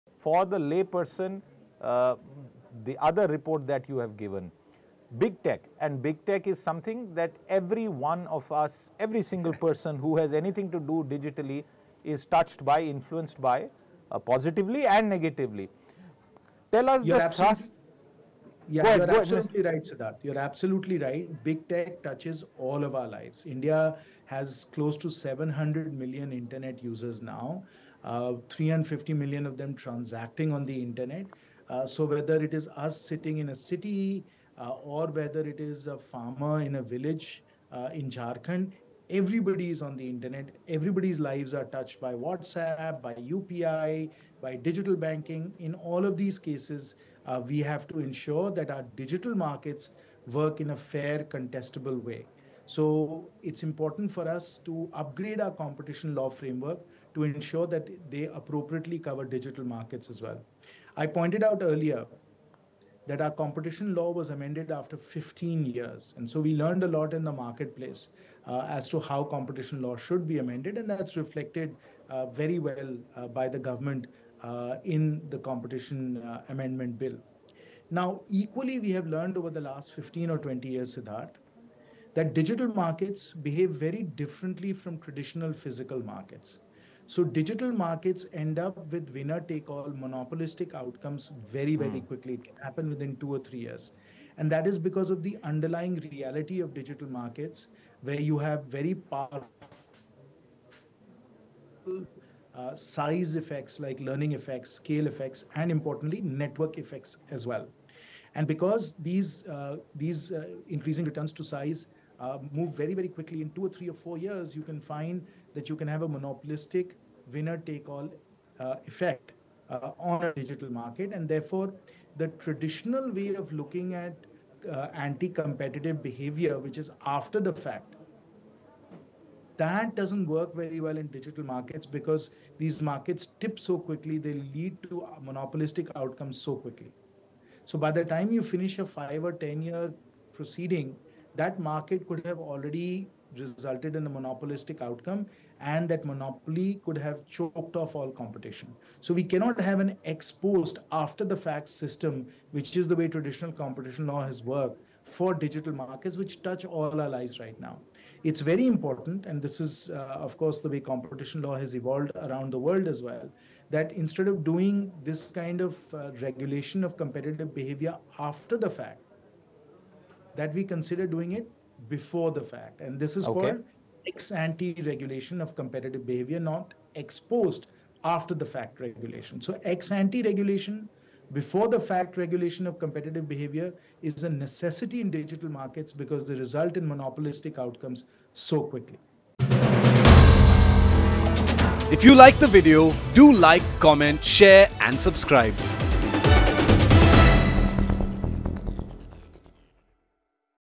Cracking Down On Big Tech Monopolies
The Parliamentary Standing Committee on Finance, headed by Jayant Sinha, Chairperson of the Standing Committee on Finance, Parliament of India and BJP Lok Sabha Member of Parliament from Hazaribagh, Jharkhand has recommended the government bring a Digital Competition Act to ensure a fair, transparent and contestable digital ecosystem, taking note of the “anti-competitive” practices by Big Tech companies.